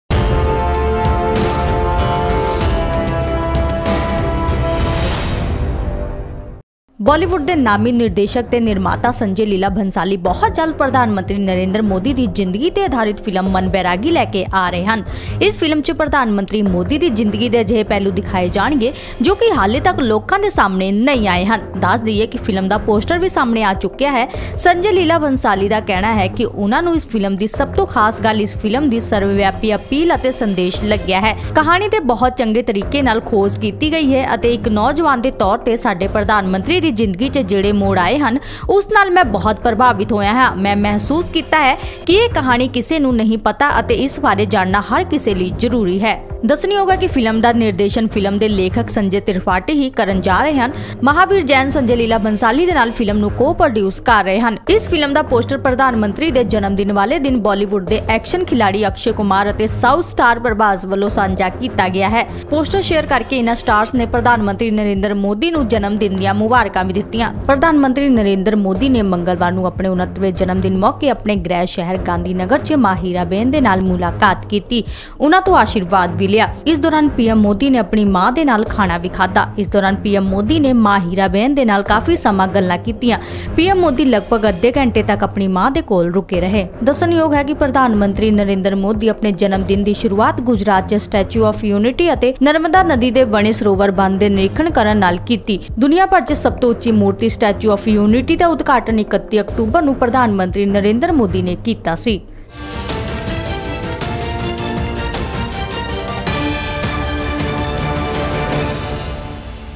Narendra Modi ਉੱਪਰ ਬਣਨ ਜਾ ਰਹੀ ਹੈ ਇਕ ਹੋਰ Film - ਜਾਣੋ ਕੌਣ ਨਿਭਾਏਗਾ ਕਿਰਦਾਰ
narendra modi Latest news
narendra modi news film news
narendra modi  birthday 2019
narendra modi new movie news
narendra modi  film 2019